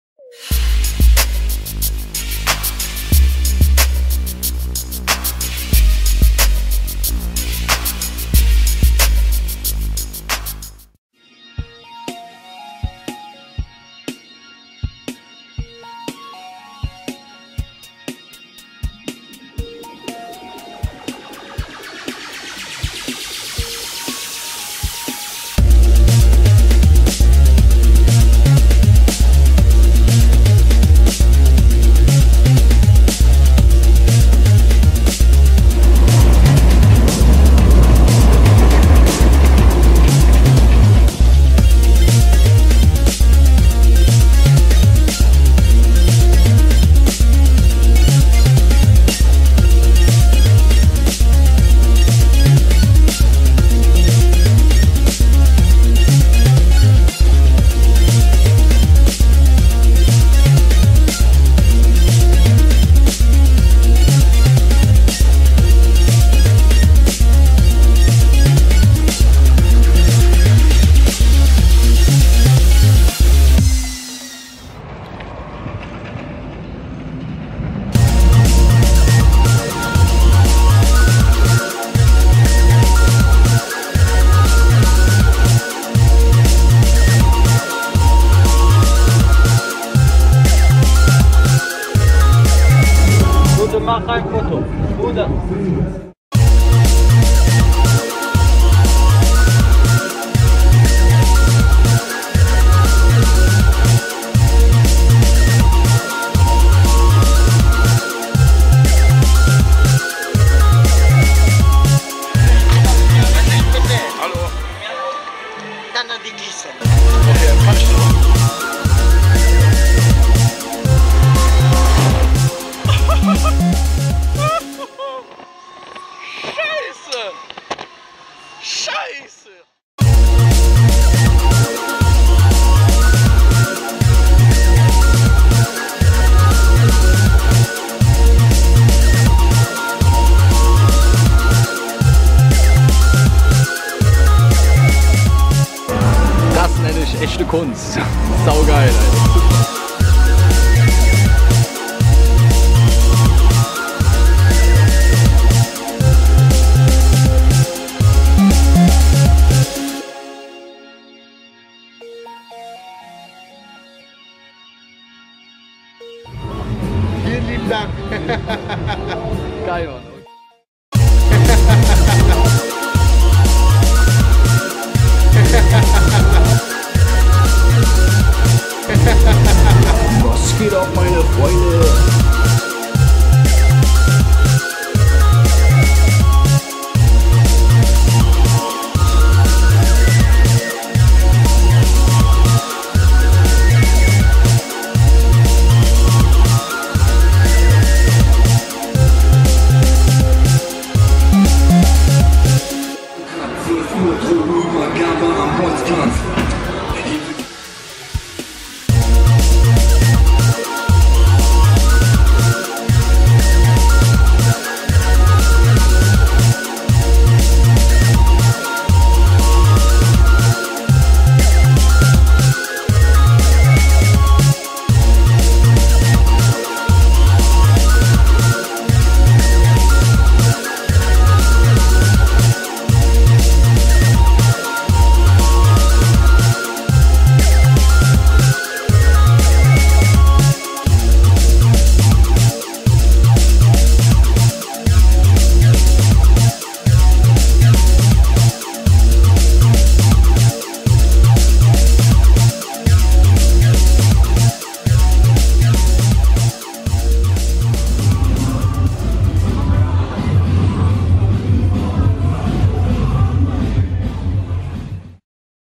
Maimesse Minden 2017 - Impressionen
Impressionen aus der Maimesse in Minden 2017 inklusive fast aller Fahrgeschäfte.

u.a. mit Teststrecke, Propeller, Frisbee, Chaos Airport, Scary House,  Europa Rad, Tom der Tiger,, Disco Swing uvm.

Die Maimesse in Minden finden vom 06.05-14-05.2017 statt.